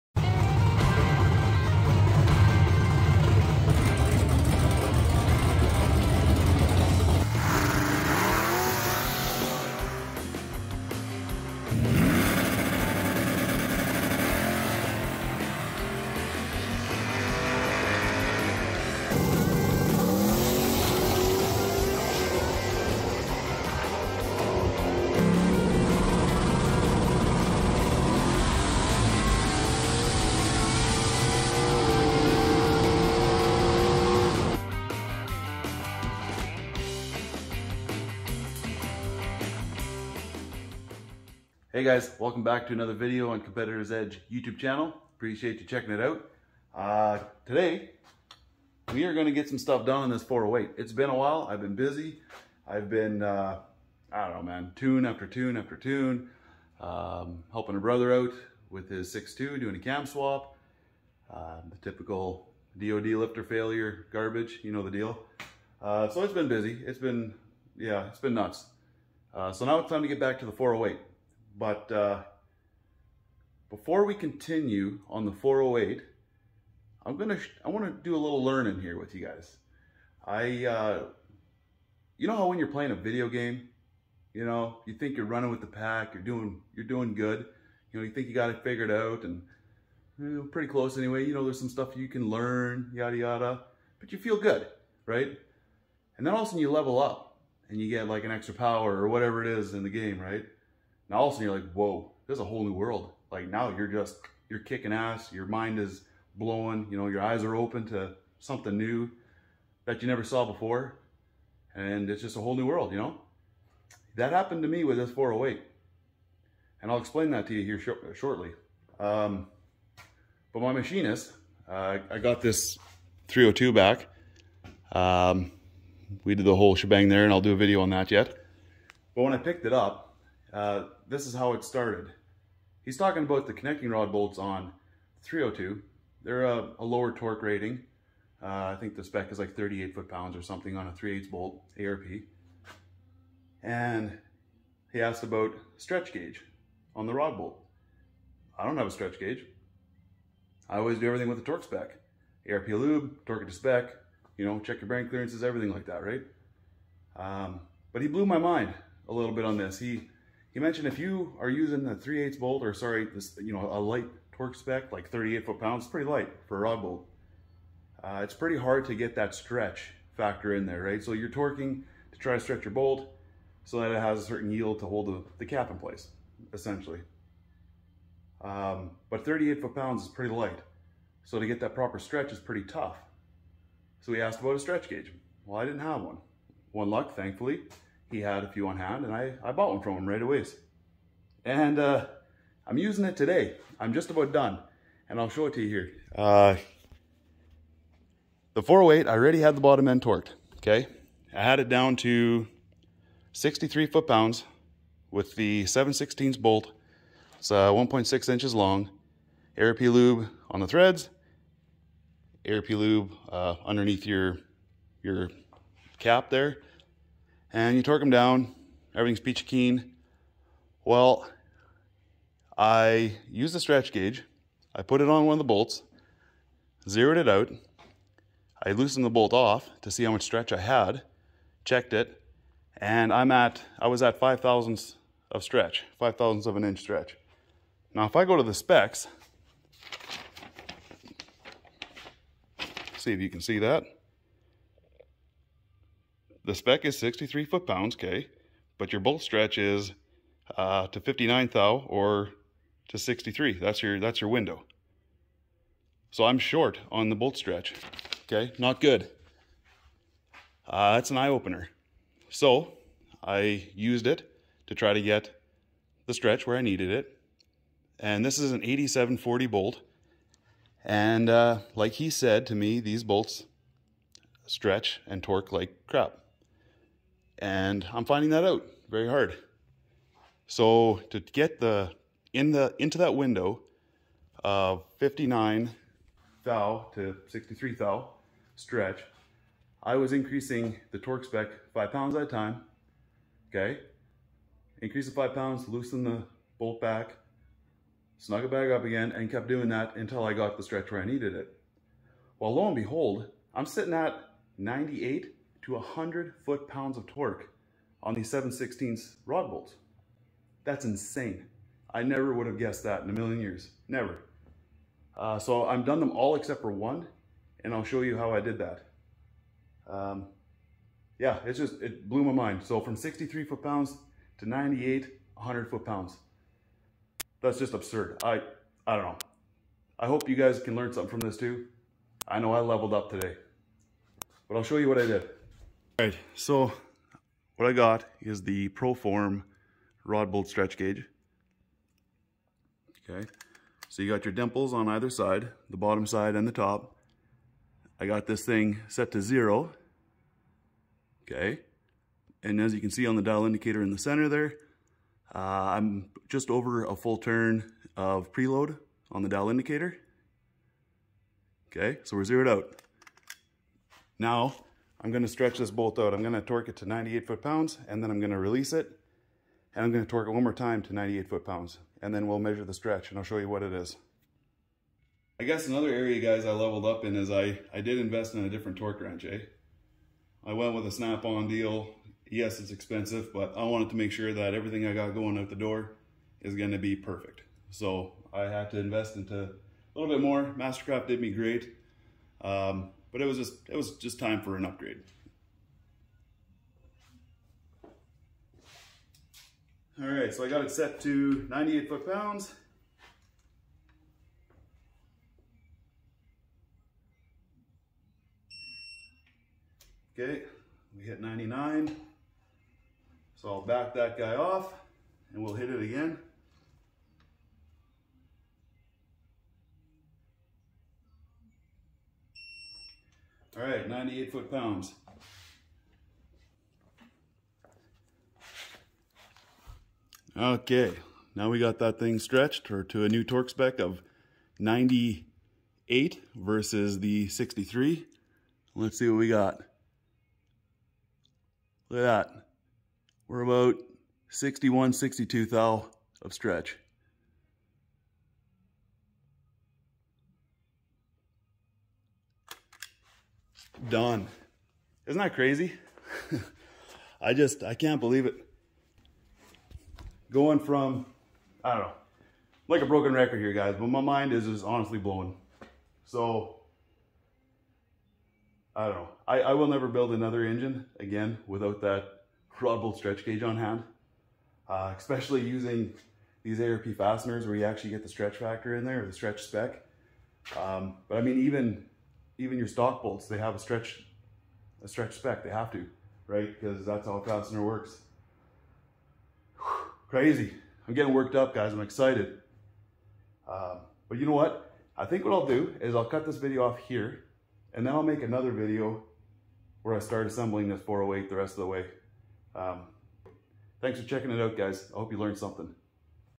How To Use A Rod Bolt Stretch Gauge!?
Learned a new way to torque down connecting rod bolts!?